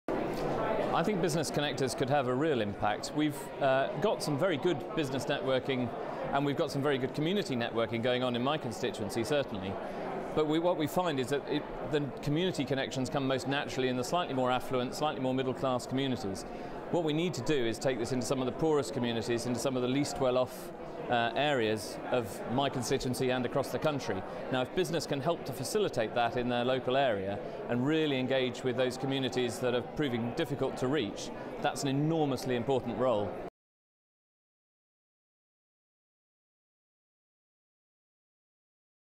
Martin Horwood, MP for Cheltenham, talks about BITC's Business Connector Programme
Martin Horwood, MP for Cheltenham, talks about the difference Business Connectors could make in communities.